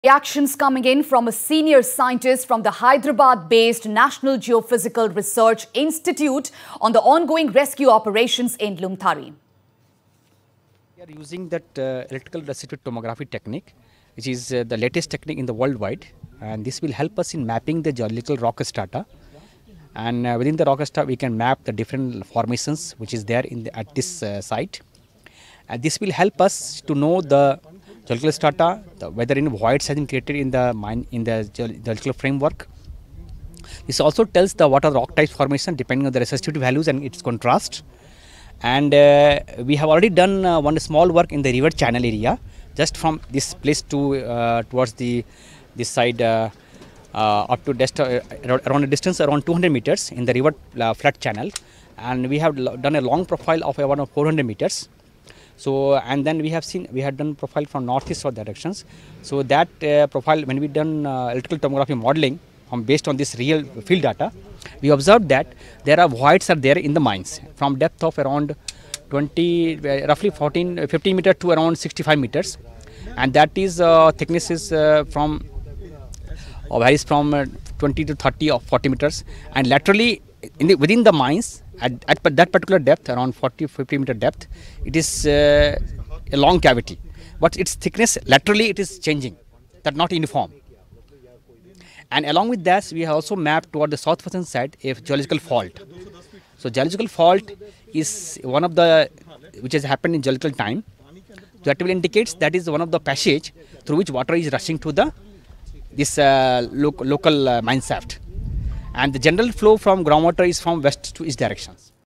MeghalayaMineRescue: NGRI Scientist on the ongoing rescue operations in disaster site
Senior Scientist from the Hyderabad based National Geophysical Reasearch Institute reacts on the ongoing rescue operations in Lumthari.